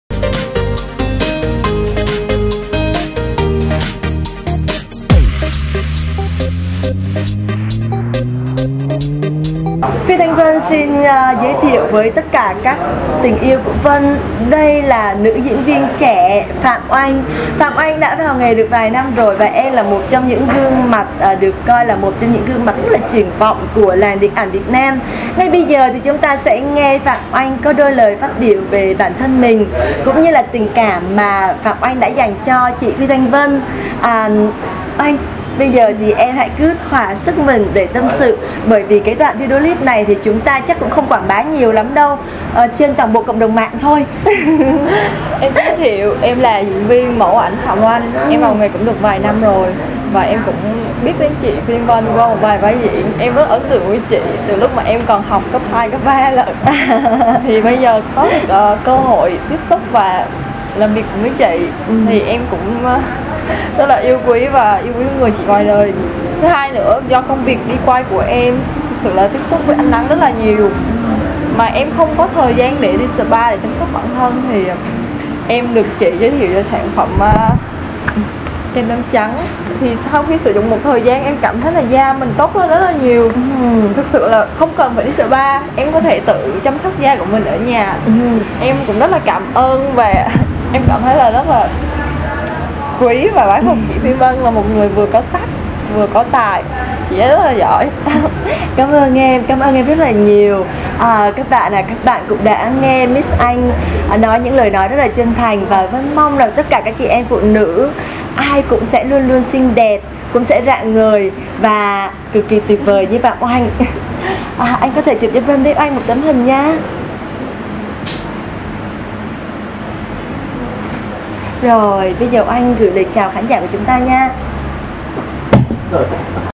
Tâm sự cùng Phi Thanh Vân: Diễn viên Phạm Oanh
Cùng Phi Thanh Vân trò chuyện về nghề, về phong cách của nghệ sĩ trong showbiz Việt.
Nơi bạn có thể tìm thấy tất cả phong cách lạ nhất của nghệ sĩ. Từ đó tạo cho mình một phong cách riêng.